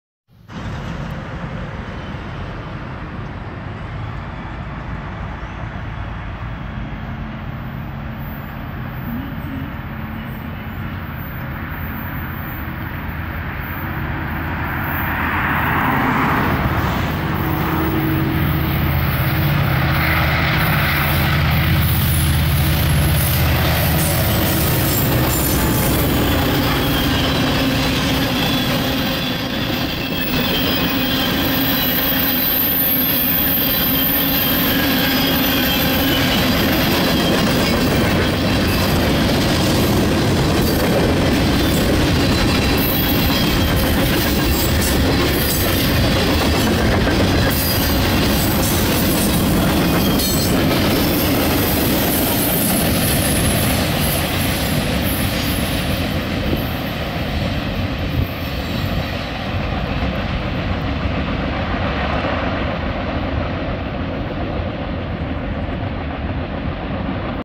(11/13/23) - The SalinasHauler rolls south through CPLick after finishing work at WS & sanjose .
(11/13/23) - After dropping outbound train cars at WarmSprings and picking up inbound cars there and at GraniteRockBerryessa , the SalinasHauler - with UP1003 and UP1456 - rolls south through CPLick . The Hauler will do some work at the quarry at Logan before returning to WatsonvilleJct .